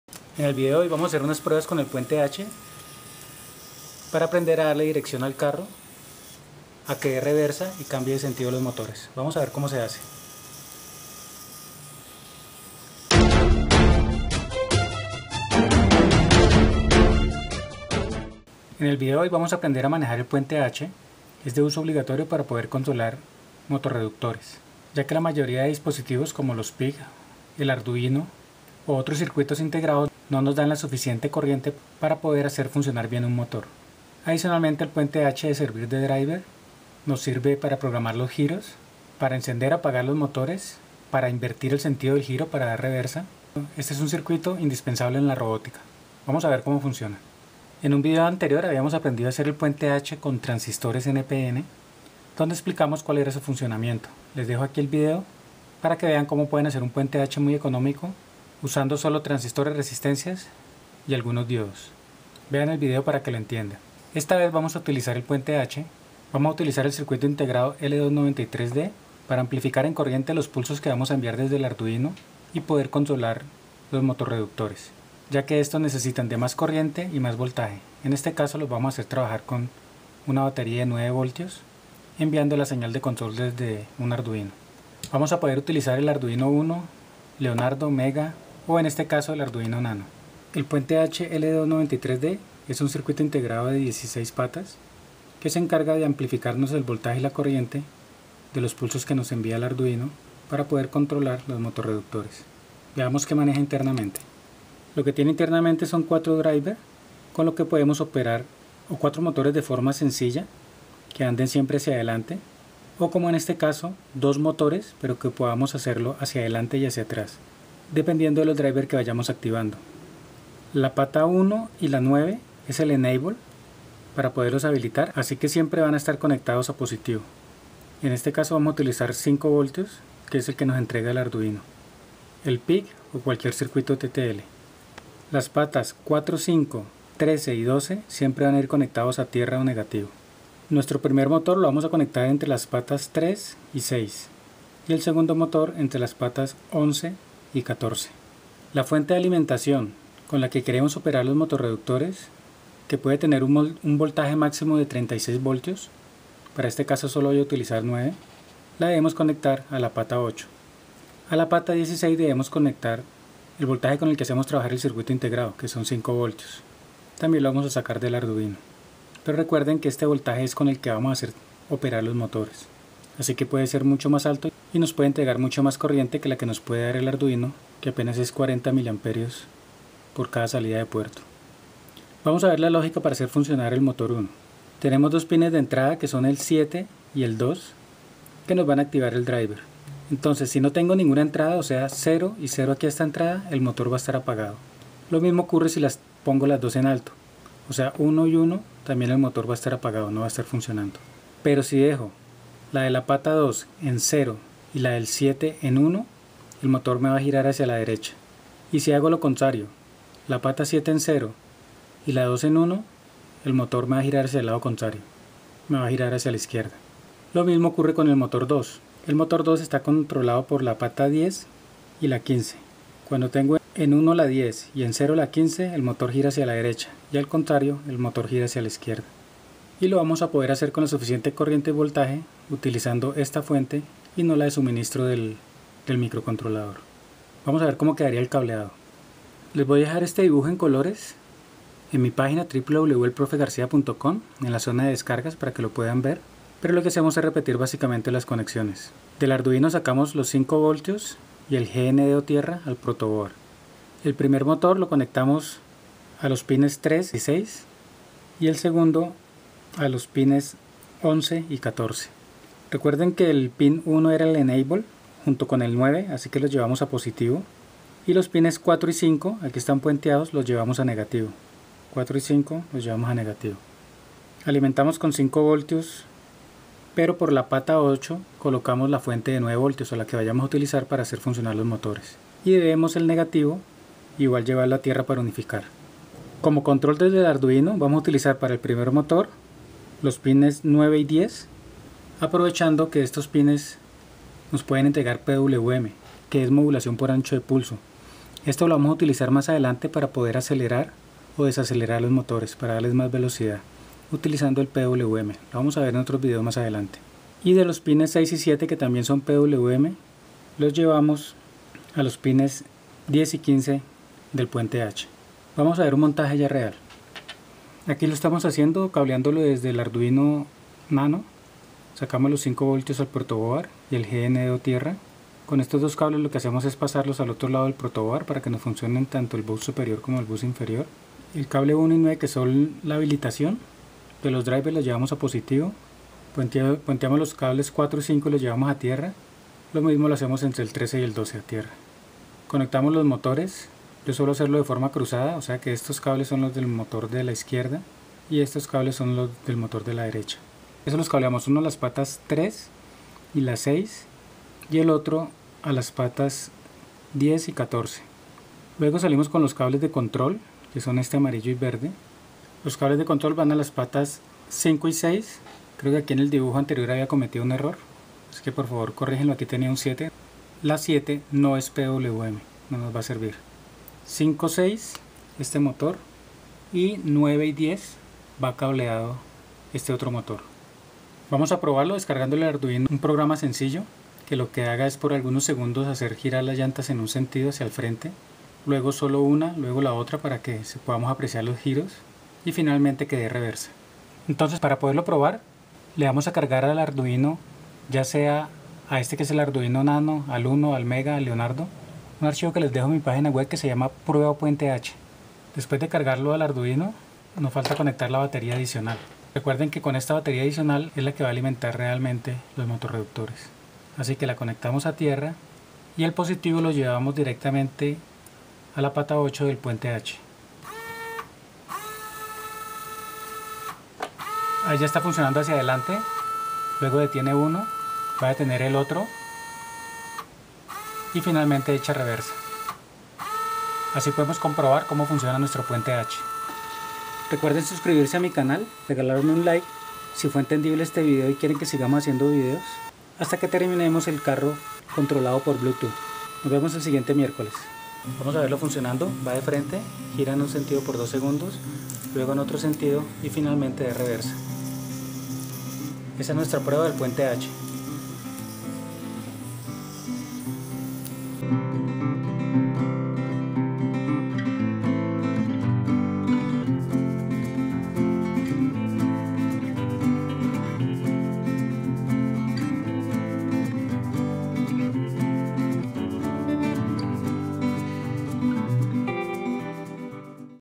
✅ Reversa y Giro para Carros y Robot (Como se hace?) Puente H
Puente H para Giro y Reversa de Robots o Carros, Tutorial de las conexiones L293D

(1) Puente H  L293D
(1) Arduino UNO R3
(1) Cables de conexion
(1) Protoboard
(2) Motorreductores 1,5Kg de fuerza, 100RPM  3-12Vdc
(2) Llantas para Motorreductores.
(1) Base bateria 9Vdc
(1) Bateria 9Vdc.
(1) Rueda Loca
Tienda Dinastía tecnologica